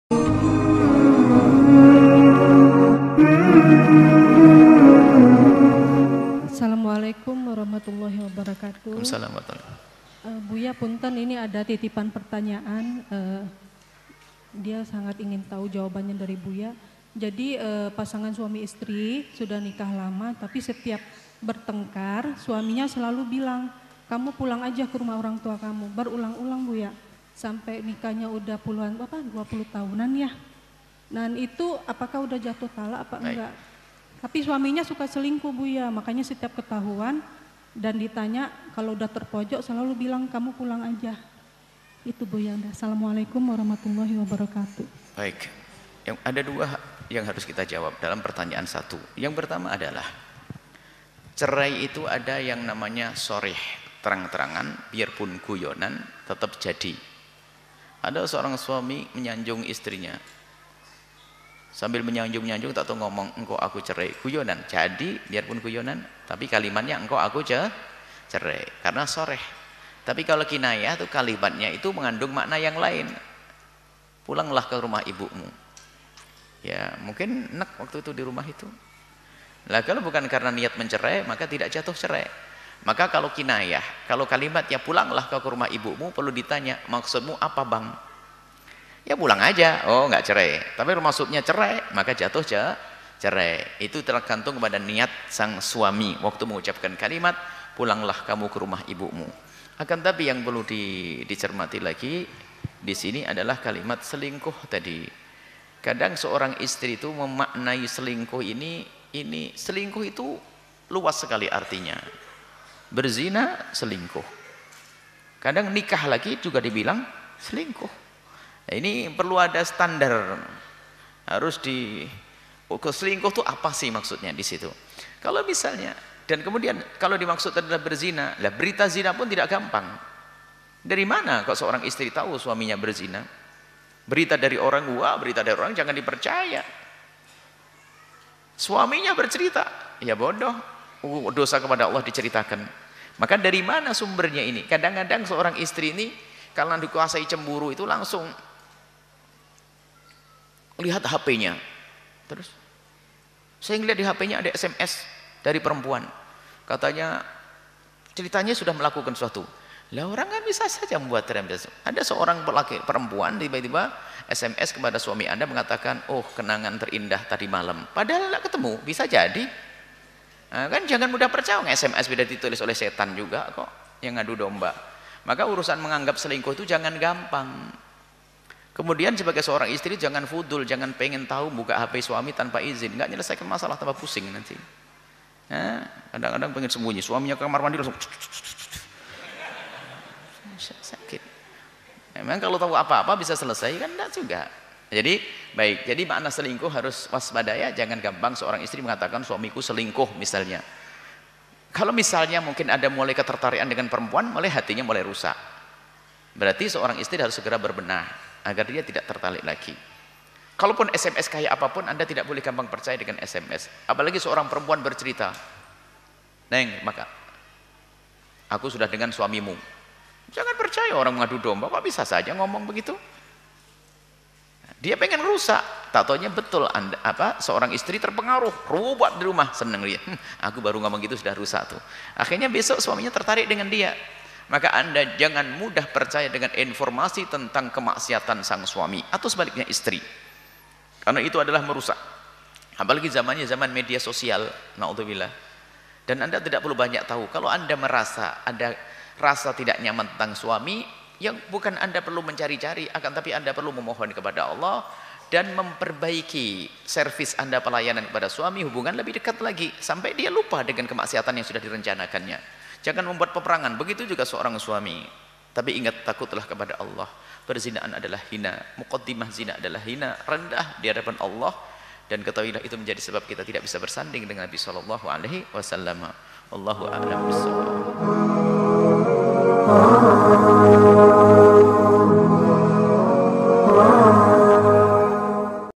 Penyebab Ketidakharmonisan Dalam Rumah Tangga Oleh Buya Yahya
Mohon Like dan Komentarnya
Bagikan Agar Manfaat